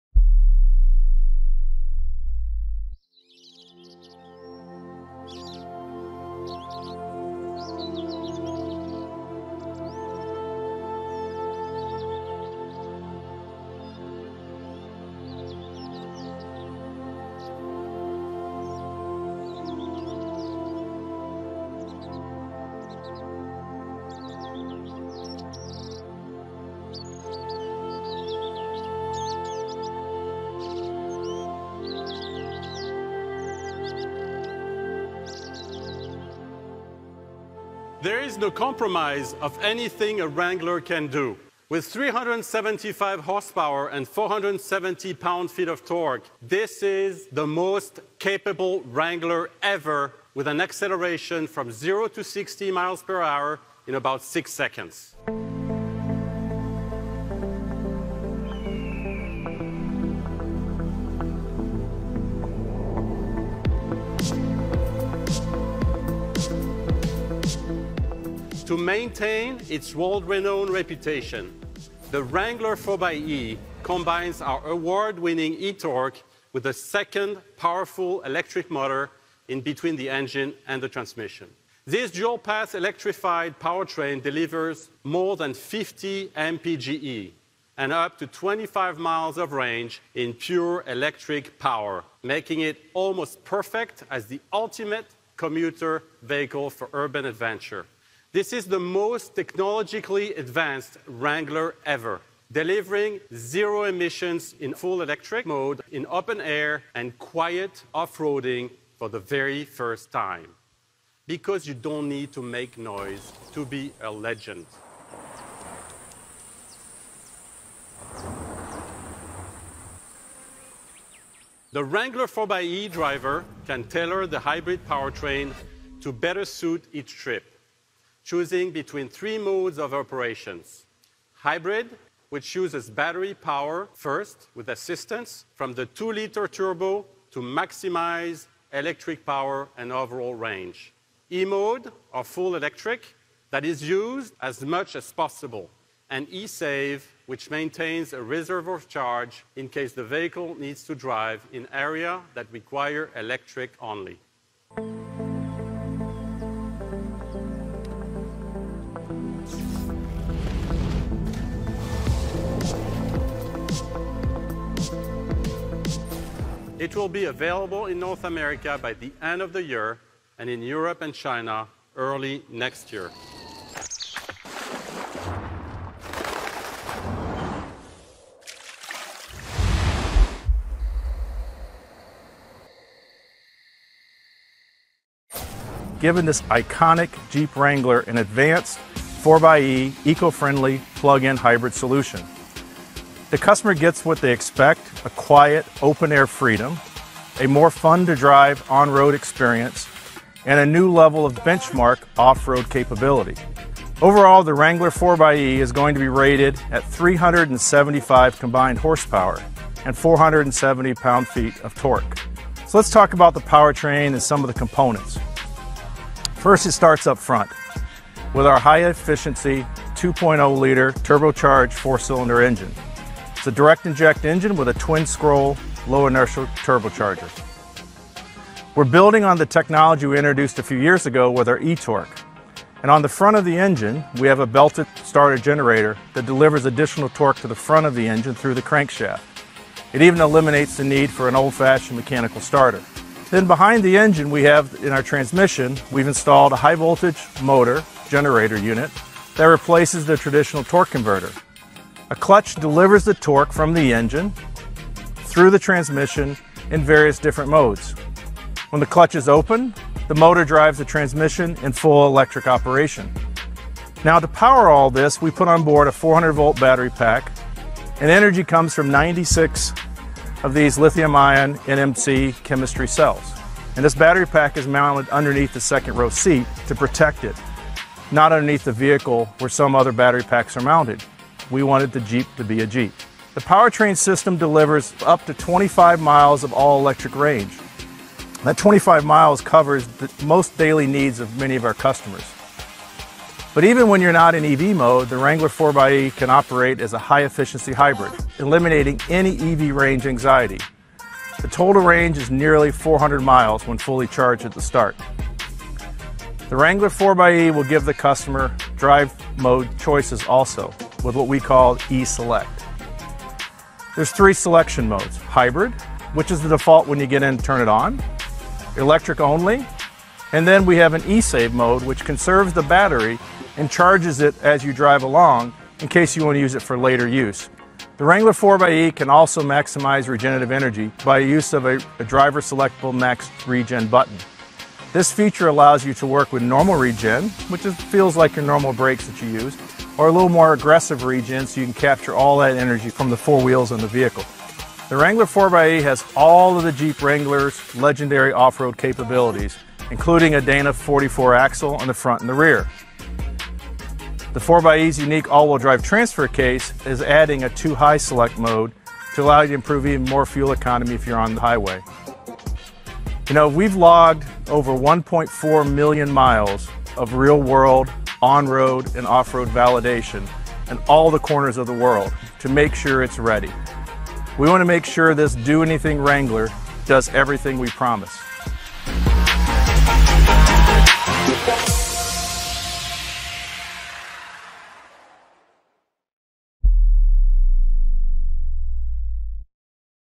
2021 NEW JEEP WRANGLER 4XE -The Most Capable Wrangler Yet - The Best Plug in Hybrid Off-road SUV ?
Jeep would like to remind you that not only is its Wrangler the most popular off-roader still on sale in the U.S., but it has more than a few tricks up its sleeve to keep the iconic Wrangler relevant in the face of Ford’s new Bronco competitor. The latest trick is the new 2021 Wrangler 4xe plug-in hybrid electric SUV.(find out more about it.)